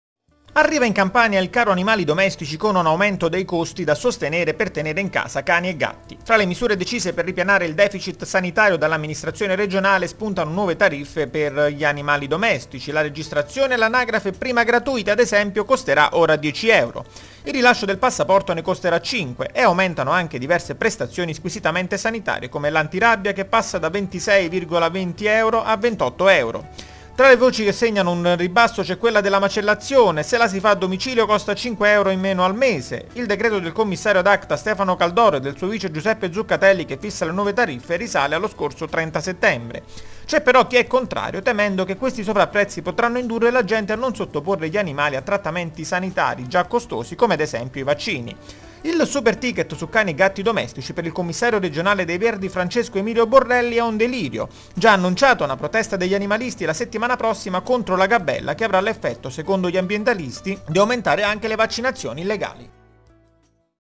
Aumentano i costi per gli animali domestici in Campania. 20-10-2010.mp4
Italia 2 Tv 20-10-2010 Aumentano i costi per gli animali domestici in Campania. Il provvedimento adottato dalla Regione fa parte del piano di rientro del deficit sanitario.